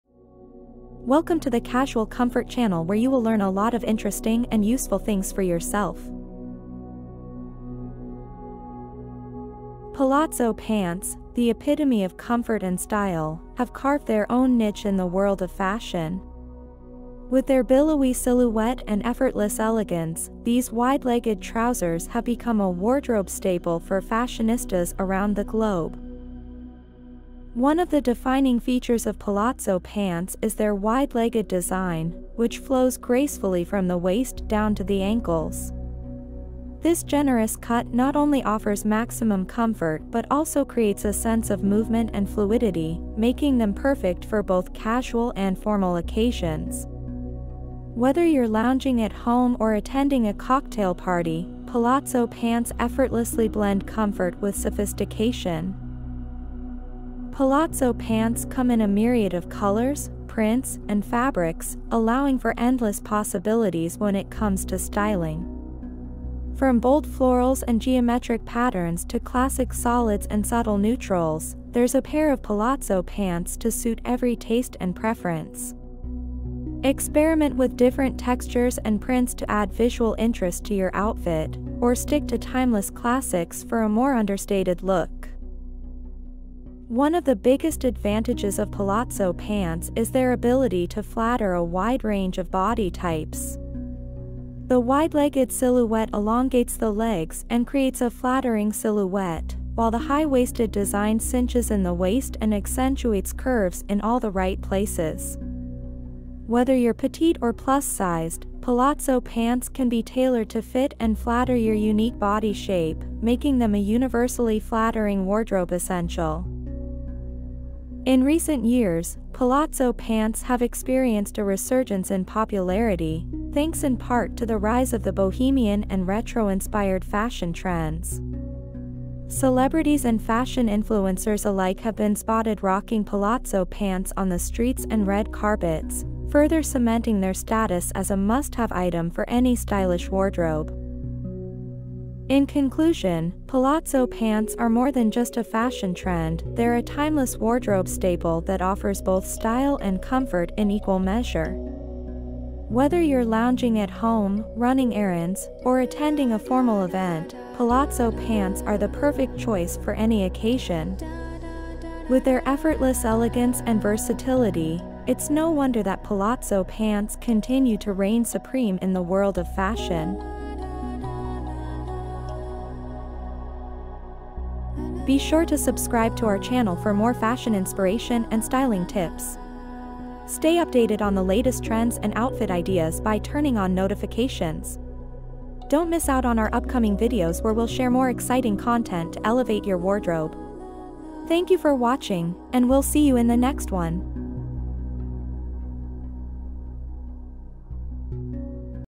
Stylish Palazzo Pants Outfits: Effortlessly Chic Fashion Inspiration
Elevate your wardrobe with our collection of trendy palazzo pants outfits! In this video, we showcase versatile and fashion-forward ways to style palazzo pants for various occasions, from casual outings to formal events. Discover how to create effortlessly chic looks with these flowy and flattering bottoms, whether paired with crop tops, fitted blazers, or cozy sweaters. With their wide-leg silhouette and comfortable fit, palazzo pants are a must-have addition to any fashionista's wardrobe. Watch now for inspiration on how to rock palazzo pants like a style icon!

10 Incredible Wide Leg Pants